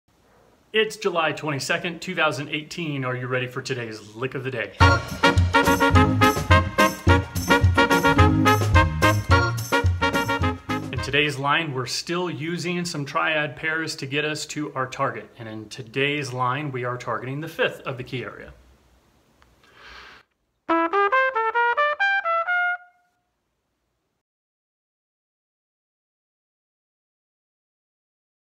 Jazz Trumpet | Lick of the Day 7.22
No fluff. A lick of the day for any Jazz musician to check out. Want to know how to use it?